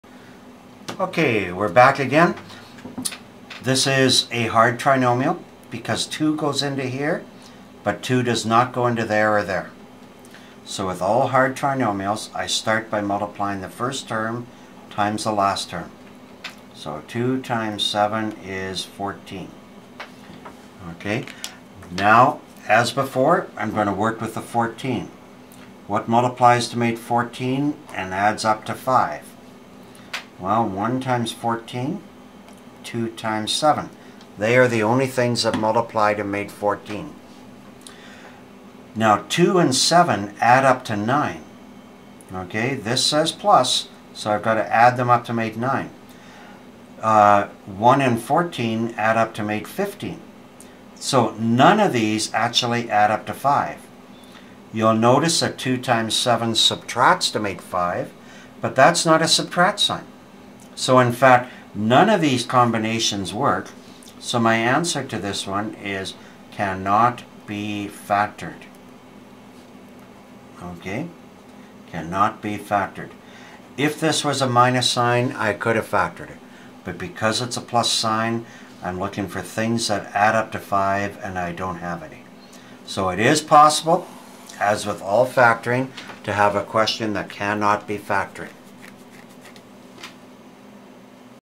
Cannot be factored?
Can all trinomials be factored?  This video is the 14th out of 16 in the Factoring series. The other three series are "Solving Equations", "Word Problems" and "Graphing".